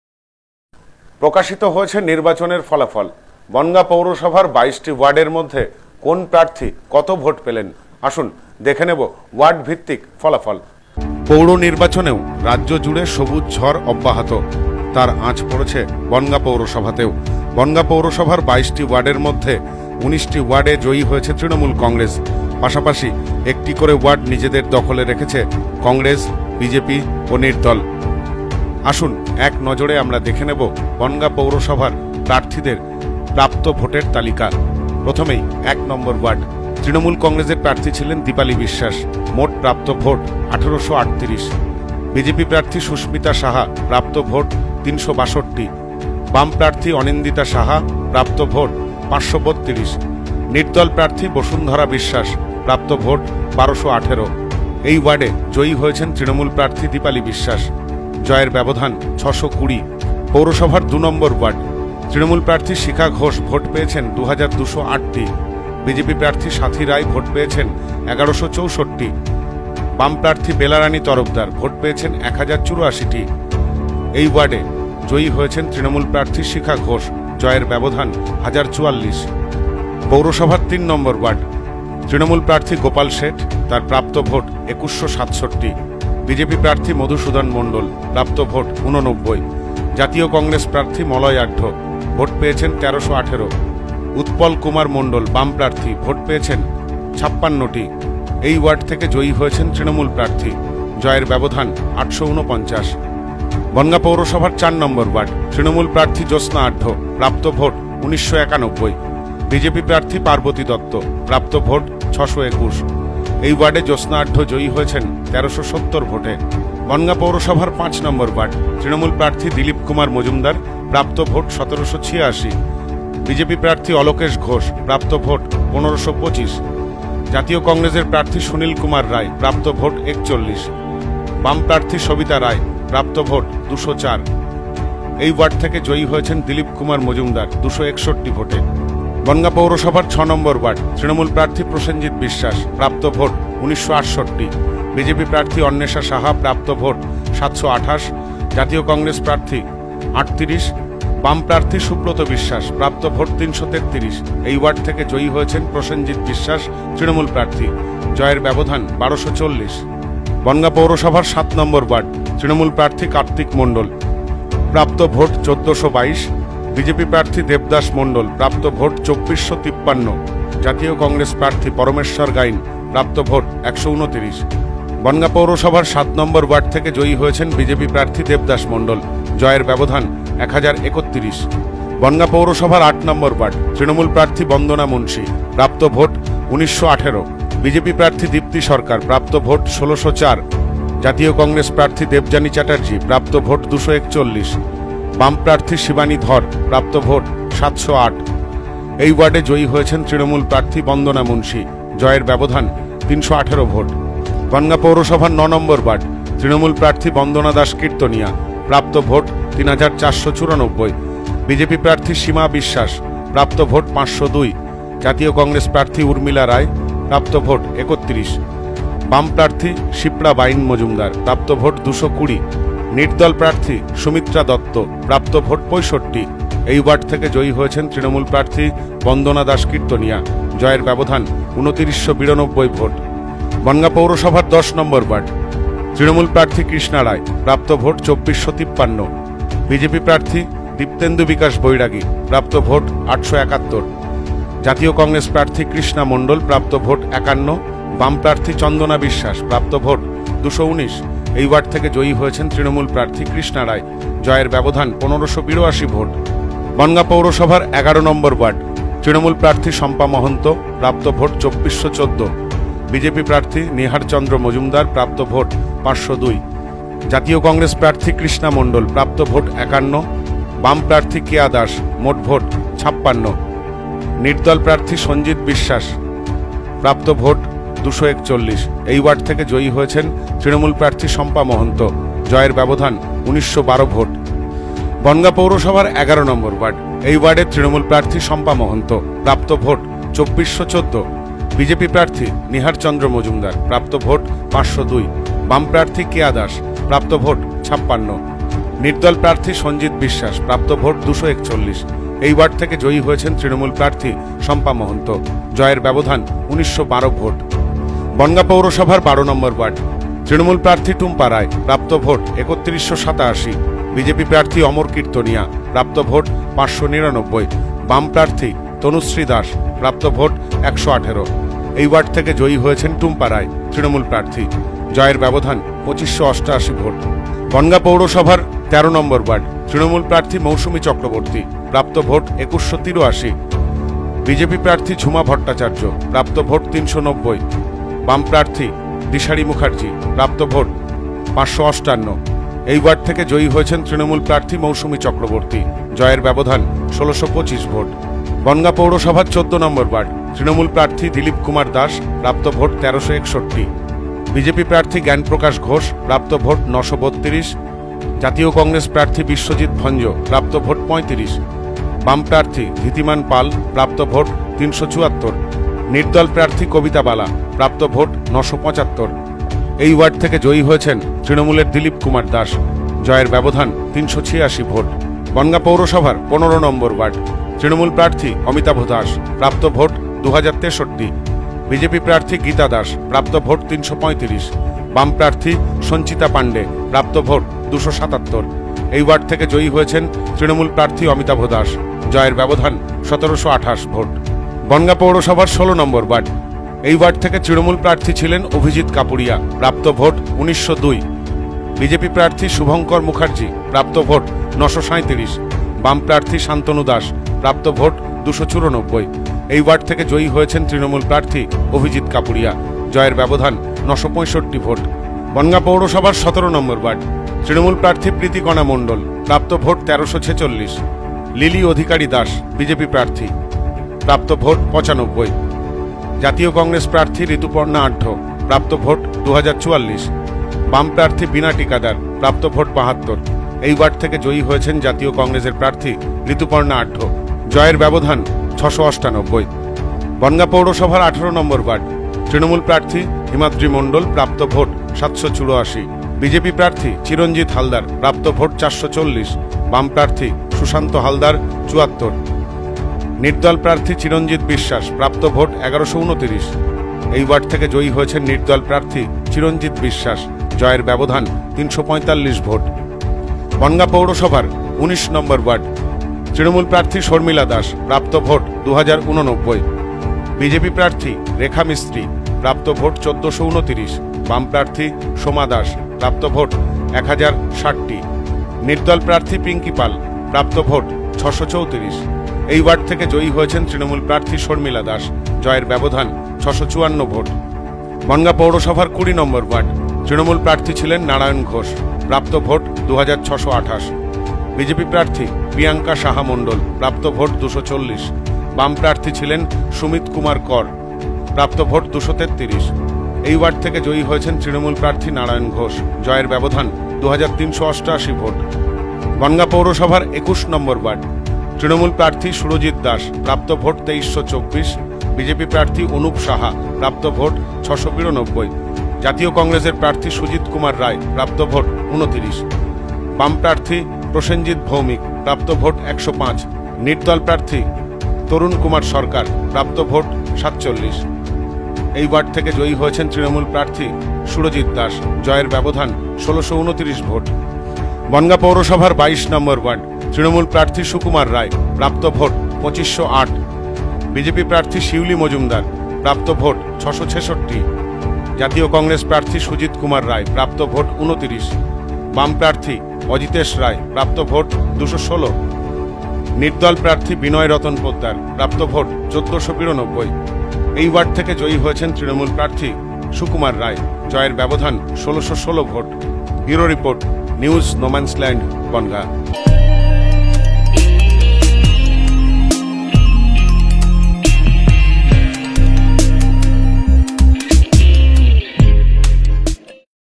বনগাঁ পৌরসভার ২২ টি ওয়ার্ডের ওয়ার্ড ভিত্তিক ফলাফল।
বনগাঁ পৌরসভার ২২ টি ওয়ার্ডে কোন প্রার্থী কত ভোট পেলেন? আসুন দেখে নেব ওয়ার্ড ভিত্তিক ফলাফল।